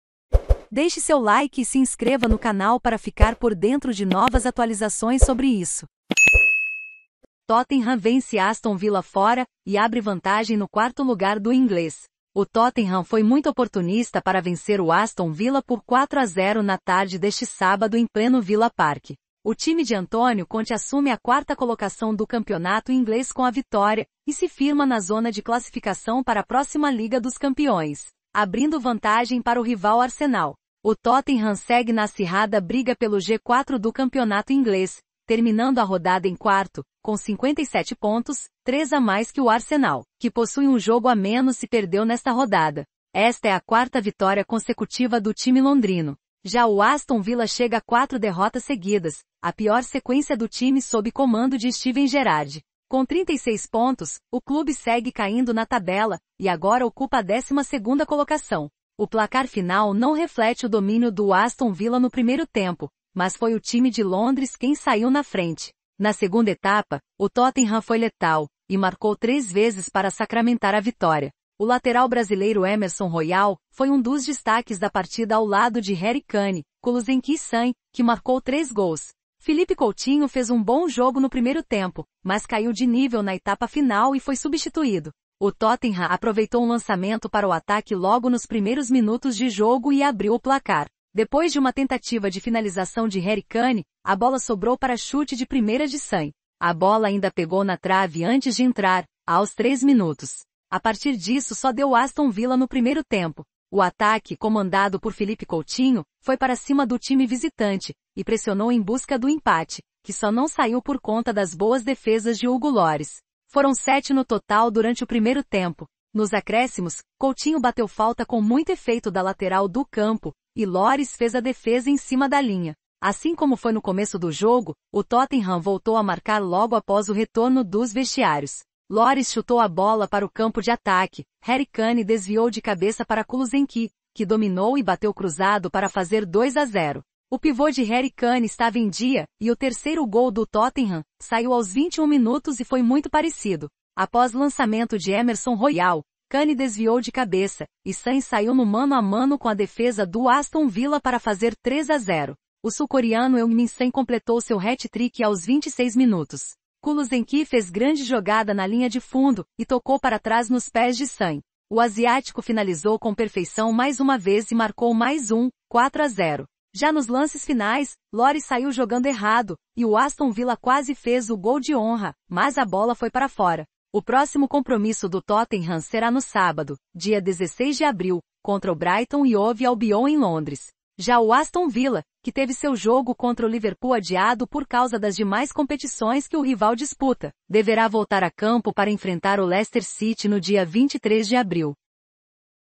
TOTTENHAM VENCE ASTON VILLA FORA E ABRE VANTAGEM NO 4º LUGAR DO INGLÊS
O Tottenham foi muito oportunista para vencer o Aston Villa por 4 a 0 na tarde deste sábado em pleno Villa Park. O time de Antonio Conti assume a quarta colocação do Campeonato Inglês com a vitória e se firma na zona de classificação para a próxima Liga dos Campeões. abrindo vantagem para o rival Arsenal. O Tottenham segue na acirrada briga pelo G4 do Campeonato Inglês. terminando a rodada em quarto. com 57 pontos. três a mais que o Arsenal. que possui um jogo a menos e perdeu nesta rodada. Esta é a quarta vitória consecutiva do time londrino. Já o Aston Villa chega a quatro derrotas seguidas. a pior sequência do time sob comando de Steven Gerrard. Com 36 pontos. o clube segue caindo na tabela e agora ocupa a 12ª colocação. O placar final não reflete o domínio do Aston Villa no primeiro tempo. mas foi o time de Londres quem saiu na frente. Na segunda etapa. o Tottenham foi letal e marcou três vezes para sacramentar a vitória. O lateral brasileiro Emerson Royal foi um dos destaques da partida ao lado de Harry Kane. Kulusevski e Son. que marcou três gols. Philippe Coutinho fez um bom jogo no primeiro tempo. mas caiu de nível na etapa final e foi substituído. O Tottenham aproveitou um lançamento para o ataque logo nos primeiros minutos de jogo e abriu o placar. Depois de uma tentativa de finalização de Harry Kane. a bola sobrou para chute de primeira de Son. A bola ainda pegou na trave antes de entrar. aos 3min. A partir disso só deu Aston Villa no primeiro tempo. O ataque. comandado por Philippe Coutinho. foi para cima do time visitante e pressionou em busca do empate. que só não saiu por conta das boas defesas de Hugo Lloris. Foram sete no total durante o primeiro tempo. Nos acréscimos. Coutinho bateu falta com muito efeito da lateral do campo e Lloris fez a defesa em cima da linha. Assim como foi no começo do jogo. o Tottenham voltou a marcar logo após o retorno dos vestiários. Lloris chutou a bola para o campo de ataque. Harry Kane desviou de cabeça para Kulusevski. que dominou e bateu cruzado para fazer 2 a 0. O pivô de Harry Kane estava em dia e o terceiro gol do Tottenham saiu aos 21 minutos e foi muito parecido. Após lançamento de Emerson Royal. Kane desviou de cabeça e Son saiu no mano a mano com a defesa do Aston Villa para fazer 3 a 0. O sul coreano Heung min Son completou seu hat trick aos 26 minutos. Kulusevski fez grande jogada na linha de fundo e tocou para trás nos pés de Son. O asiatico finalizou com perfeição mais uma vez e marcou mais um. 4 a 0. Já nos lances finais. Lloris saiu jogando errado e o Aston Villa quase fez o “gol de honra”. mas a bola foi para fora. O próximo compromisso do Tottenham será no sábado. dia 16 de abril. contra o Brighton & Hove Albion em Londres. Já o Aston Villa. que teve seu jogo contra o Liverpool adiado por causa das demais competições que o rival disputa. deverá voltar a campo para enfrentar o Leicester City no dia 23 de abril.